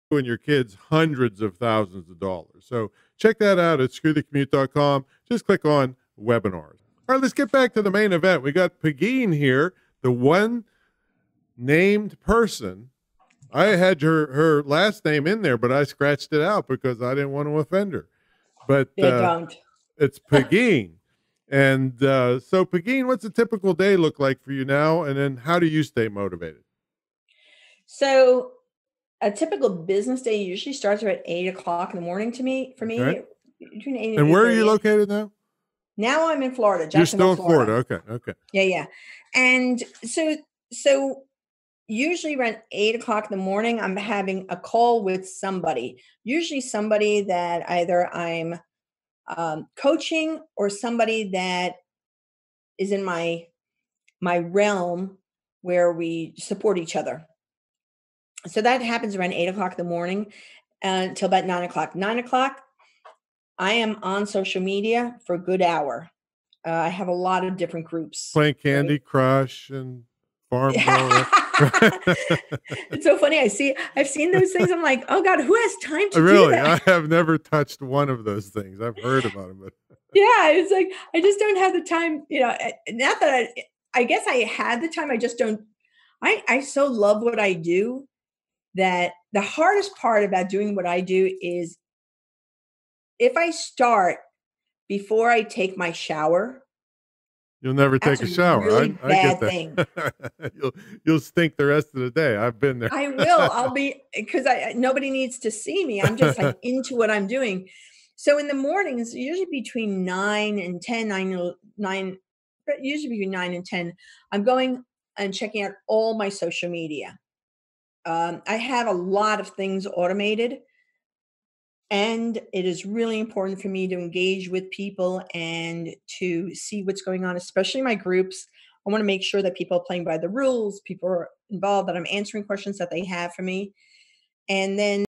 A typical day for Pegine and how she stays motivated
Pegine is in the motivational speaker Hall of Fame. She was named minority business entrepreneur. Top Business of the Year. And she's been interviewed by The Wall Street Journal, Forbes, CNN and Univision. She's the founder of the Power Women Global for women and leadership and business with over 5000 members. She's also the founder of Power Women Pro for Women, speakers, experts and authorities that has over 600 global members.

03:11 Tom's introduction to Pegine Echevarria

09:30 Guys want to be in her group!

12:56 Gang member? Tom is afraid of her

22:45 Looking for Mrs. B

25:43 Transitioning to being an entrepreneur

39:39 Sponsor message

41:12 A typical day for Pegine and how she stays motivated

Pegine's books

The Wordpress Ecourse. Learn how to Make World Class Websites for $20 or less.